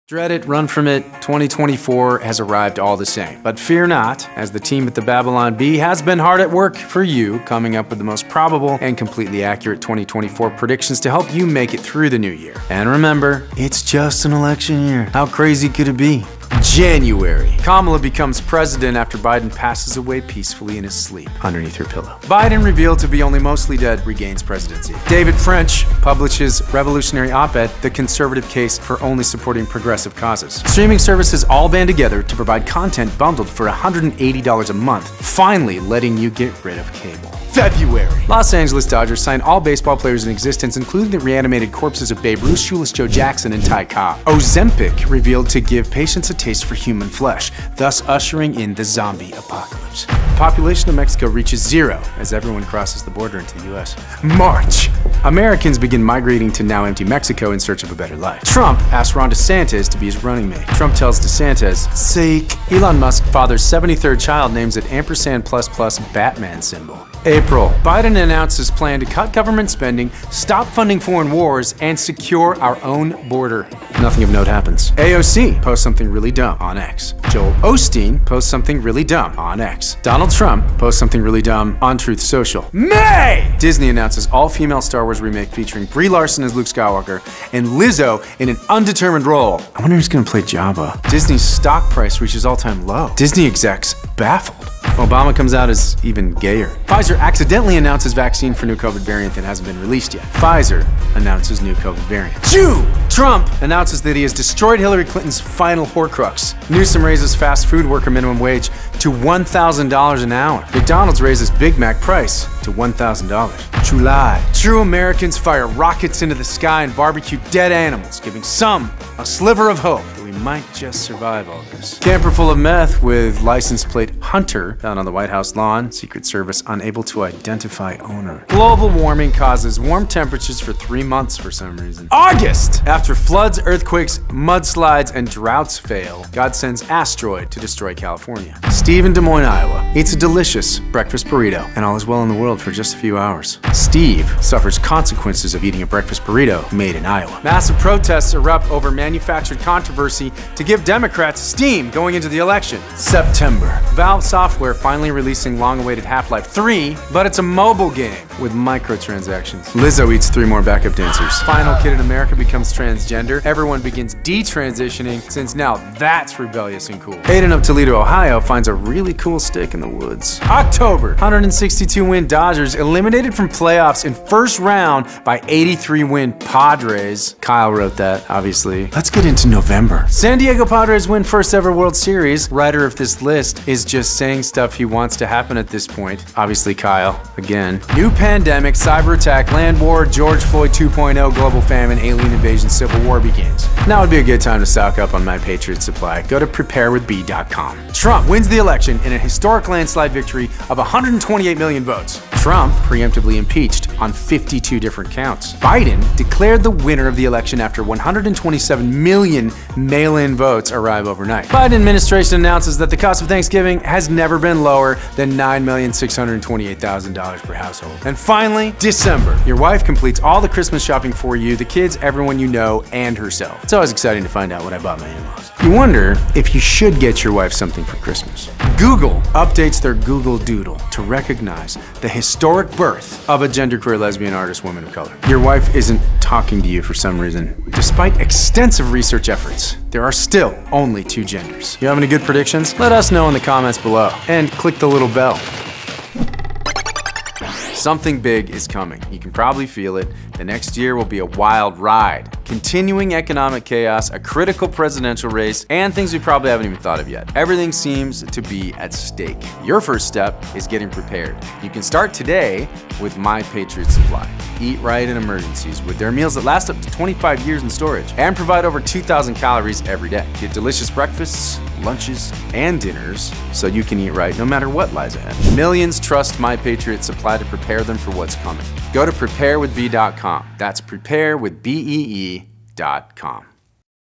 The Babylon Bee Presents: Our 100% Accurate Predictions For 2024
The Babylon Bee has been known to accurately predict the future. We have a better batting average than Benny Hinn, Nostradamus, and your local weatherman combined. So sit back and relax as we tell you with 100% certainty everything that's going to happen in 2024.

Millions trust @MyPatriotSupply to prepare them for what’s coming. Prepare today and be ready for anything